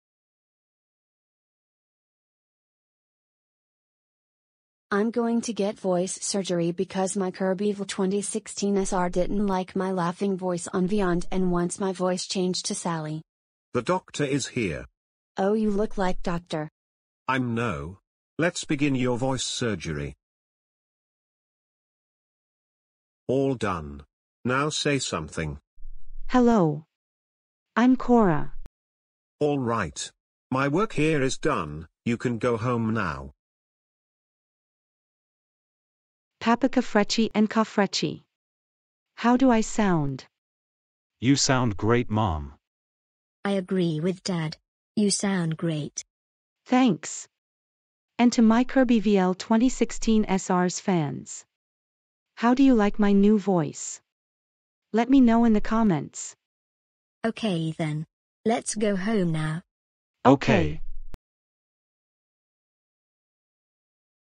Mamacoffretchi Gets Voice Surgery
After Coffretchi's Mom of New Voice Cora
Tamagotchi Voice is:
Coffretchi: Emma